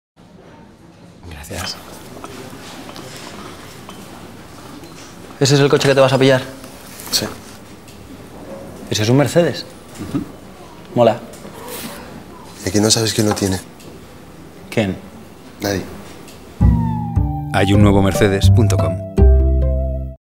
Mercedes-Benz: Spot Nuevo Clase A "Nadie"
Un chico se sorprende al descubrir que su amigo se va a comprar el nuevo Clase A.
Agencia y año de creación:  Contrapunto BBDO. Año 2012.